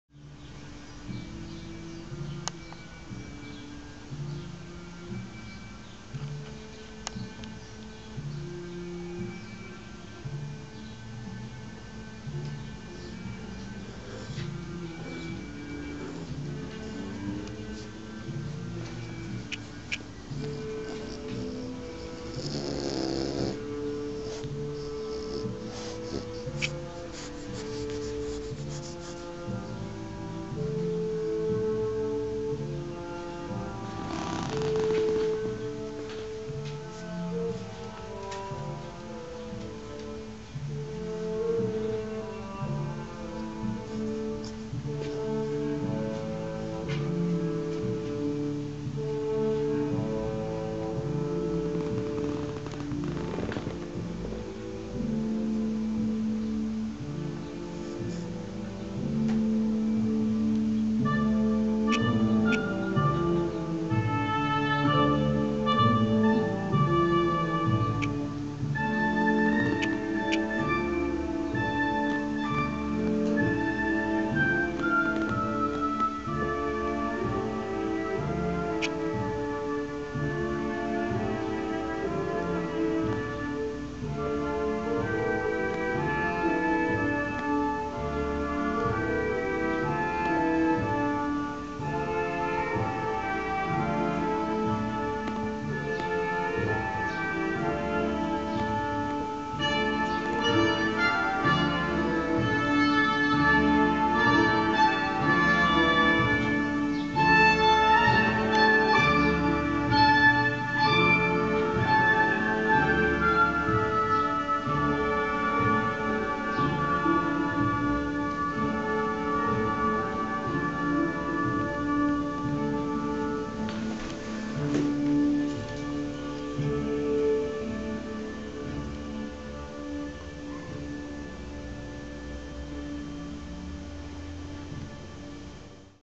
Doña Clemencia The English Bulldog Puppy - Chillin' and listening to some Mahler
Doña Clemencia just chillin' and listening to some Mahler.